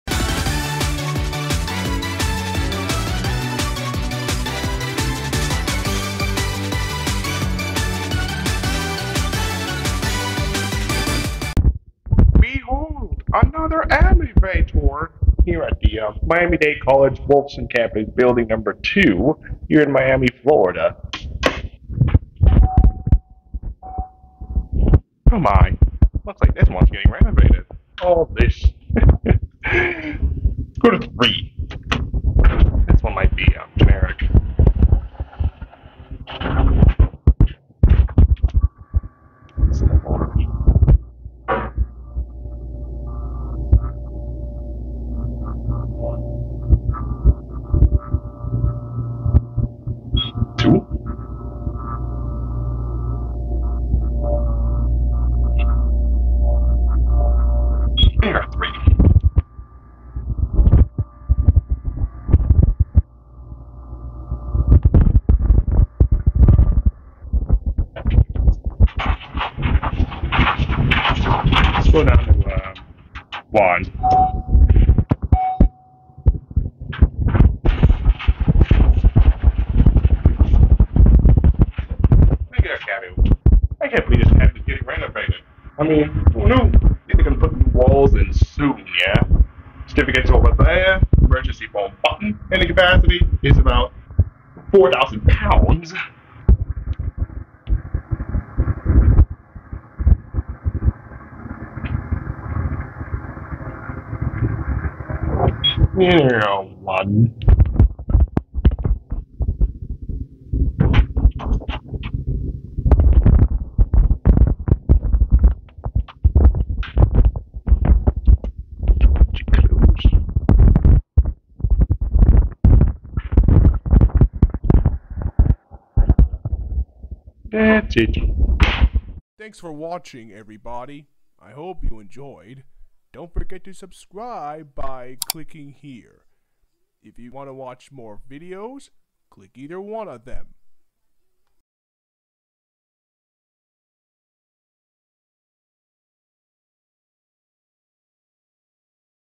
Modded Dover Hydraulic Elevator 2 @ Miami Dade College Wolfson Campus Building 2 in Miami, FL
This elevator was broken months ago but now it's working and under renovation.
Elevator Facts

Original Brand: Dover
Hoist: Traction
Fixtures: Innovation Bruiser
Type: Passenger
Capacity: 4000 lbs
Speed: 150fpm
Video Facts

Date Recorded: 8/22/24
Recording Device: Samsung Galaxy S24 Ultra
Formats: 1080p 60fps Ultra-wide angle
Other Elevator Enthusiasts: None
No spamming or inappropriate comments, please.